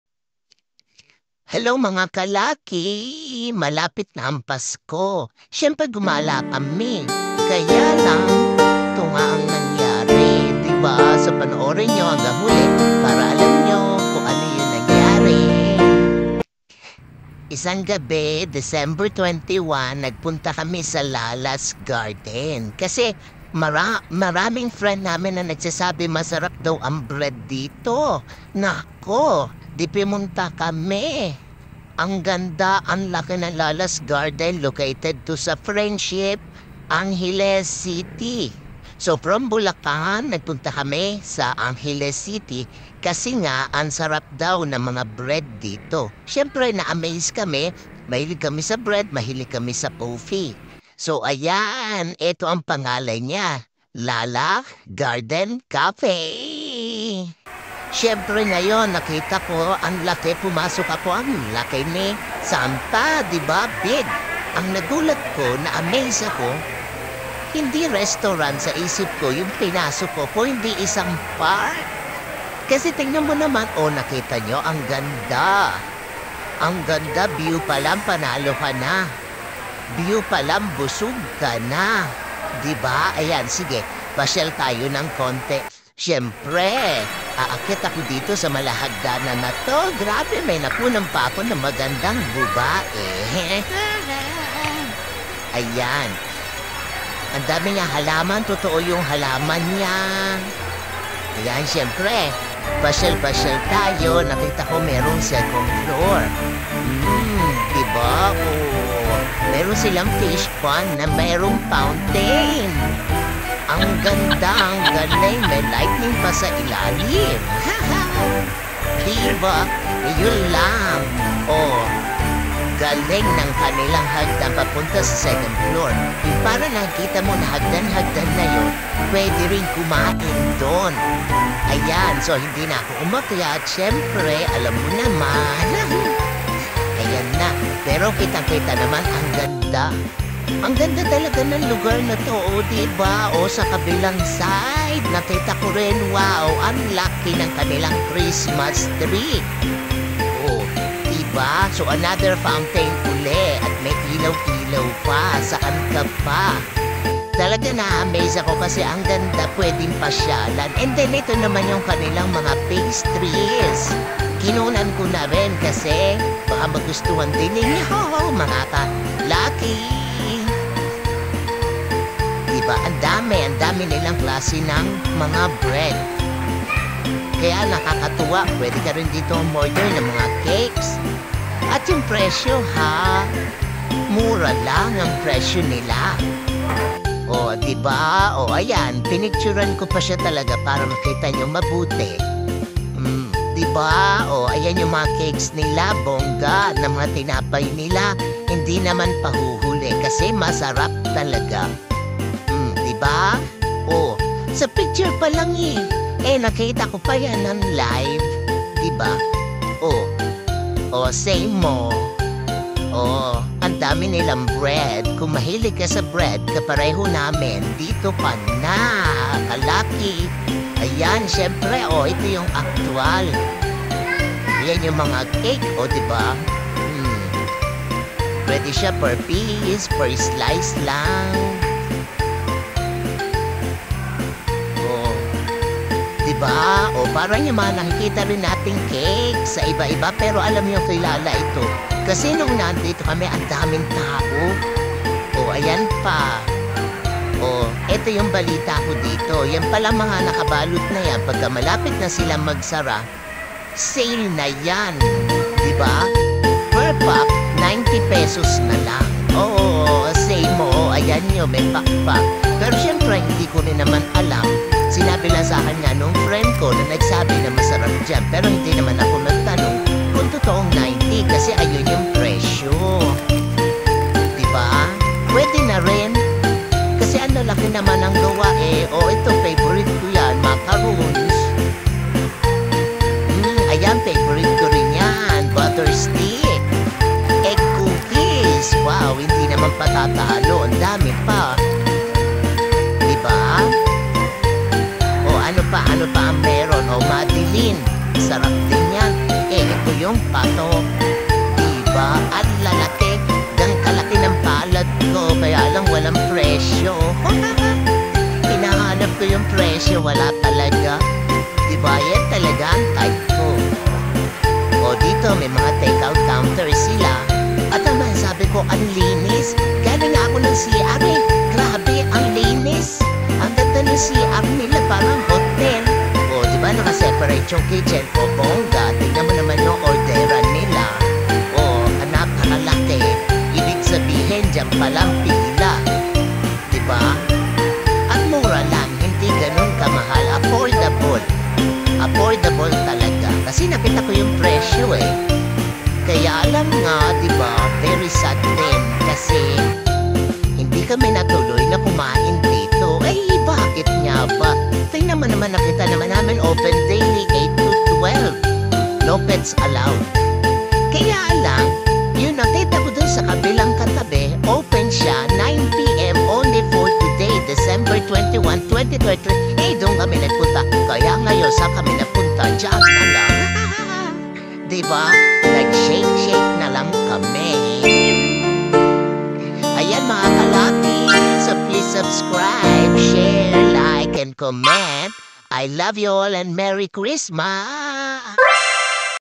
Malapit na ang PASKO, Gumala kmi kaya lang Malungkot ang Nangyari...
Hello po mga Ka Lucky share ko p sa inyo ang masaya at malungkot na nangyari sa amin nung nag decide kmi na puntahan ang LALA's Garden Cafe sa Friendship, Angeles City, recommendations ito ng friends namin masarap at maganda raw ito. Watch na lng po mga Ka Lucky hanggang dulo pray ano ba ang nangyari.